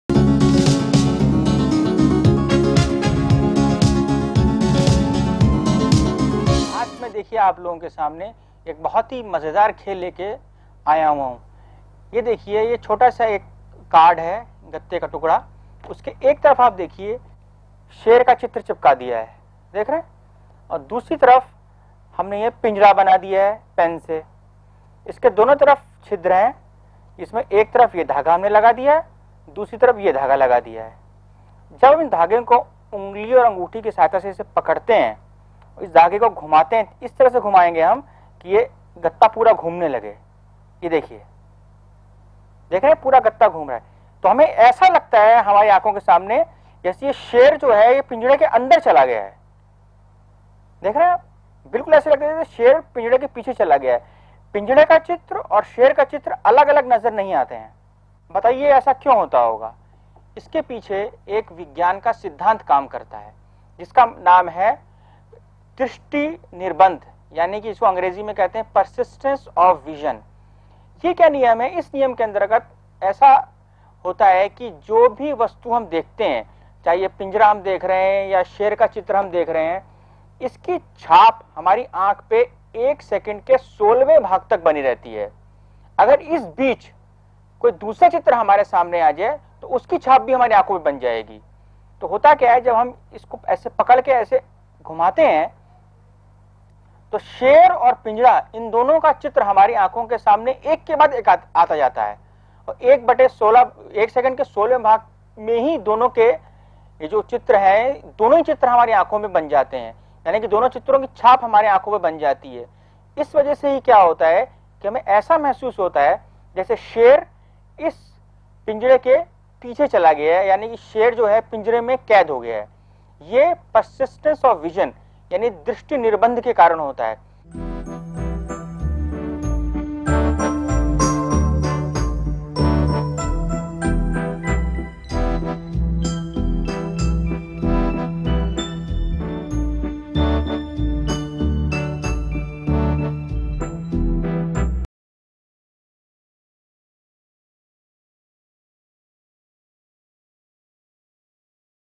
Science Corner / साईंस कॉनर्र
इस कार्यक्रम में दृष्टि निर्बंध के बारे में प्रयोग द्वारा बताया गया है।